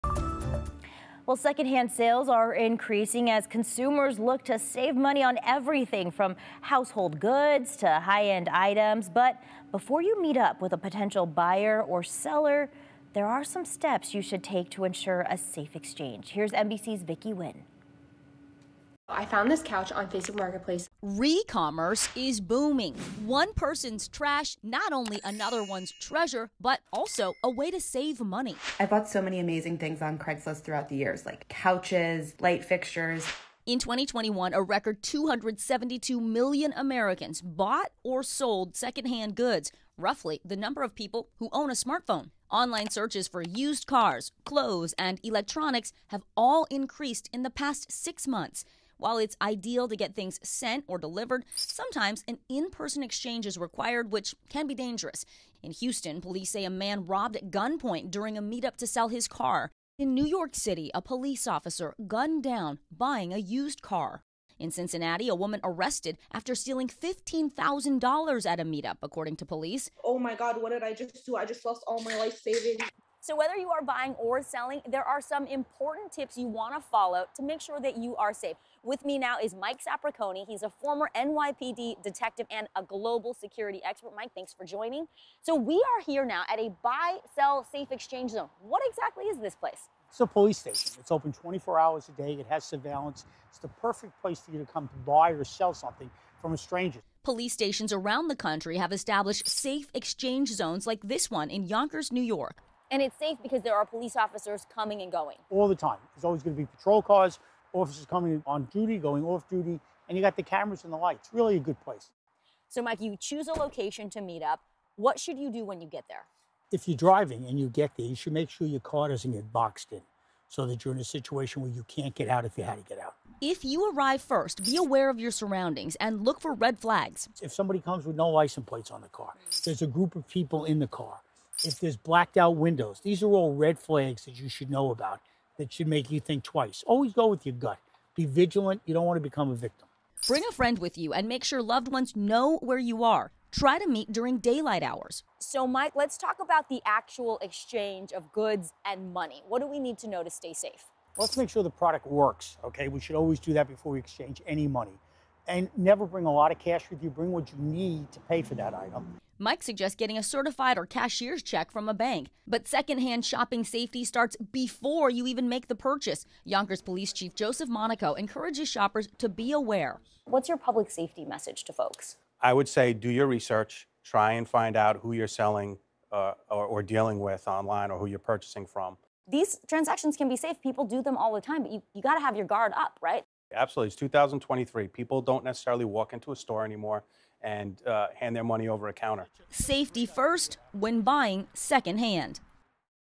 Tips to stay safe when buying or selling secondhand goods online
NBC’s Vicky Nguyen shares important safety measures to take when looking to buy or sell items online.